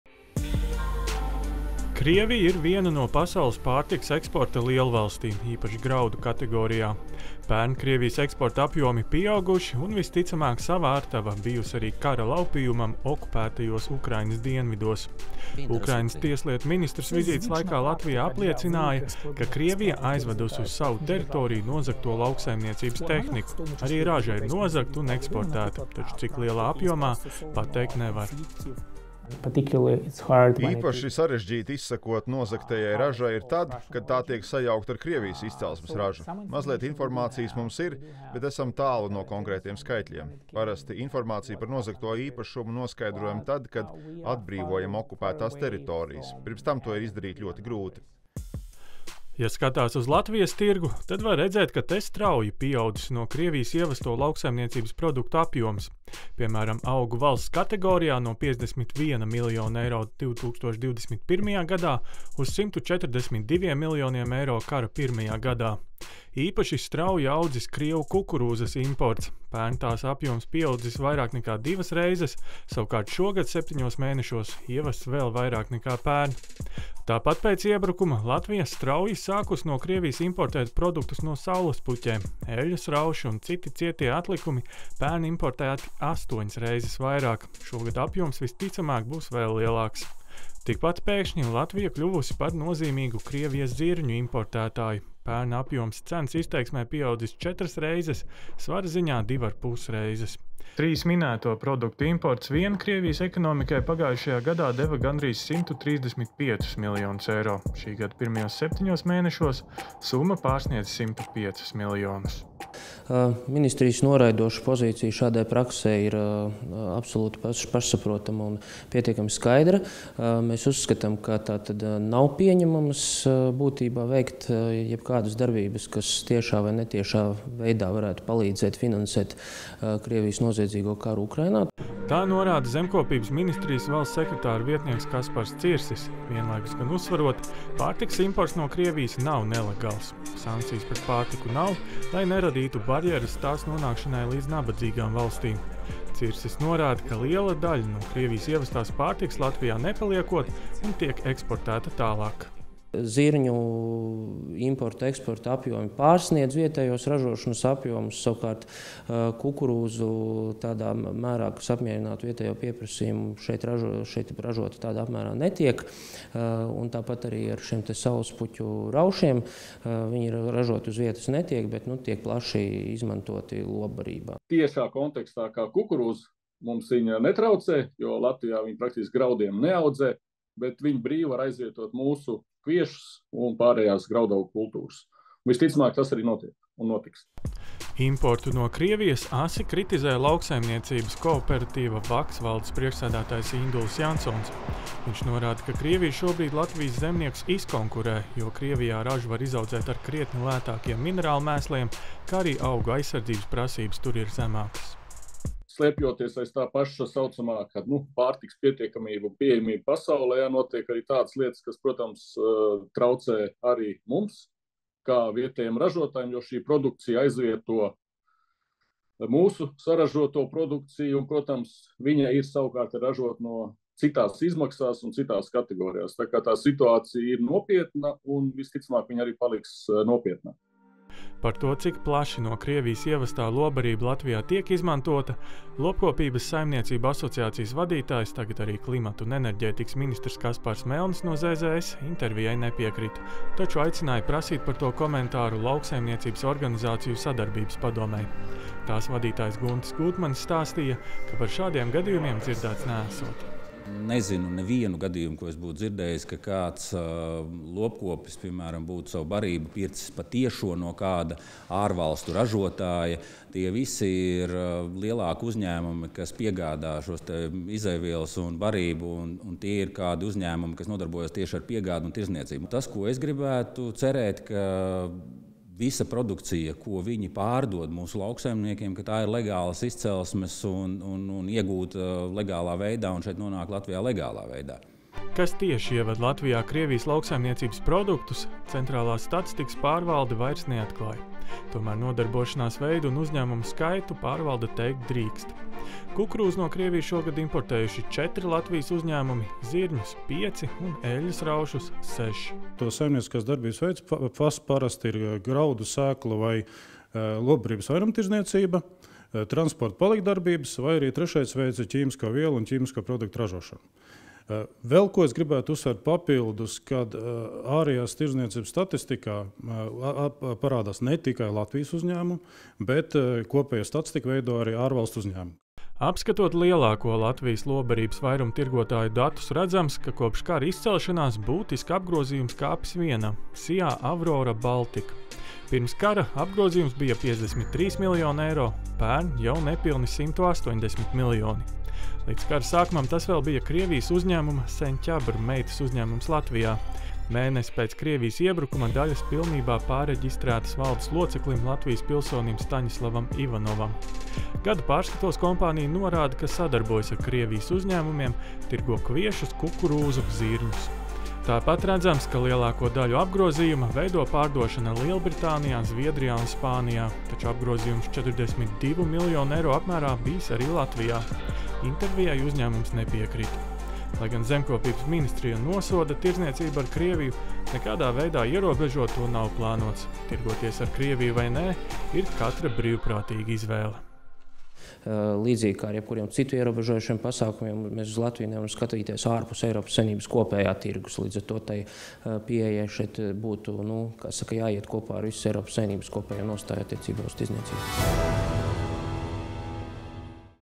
Teju trīskāršojies augu produktu imports no Krievijas
Kamēr kopumā Latvijā no Krievijas ieved mazāk preču, ir produktu kategorijas, kuras Krievijas ekonomikai tāpat ienes simtos miljonu eiro lielus ienākumus. Tā ir lopbarība, īpaši kukurūza, eļļas rauši no saulespuķu sēklām un citi lauksaimniecības produkti, kuru globālajā tirgū Krievija konkurē ar Ukrainu, un arī ar Latvijas lauksaimniekiem. Kādēļ pieaugusi lauksaimniecības produktu plūsma no agresorvalsts?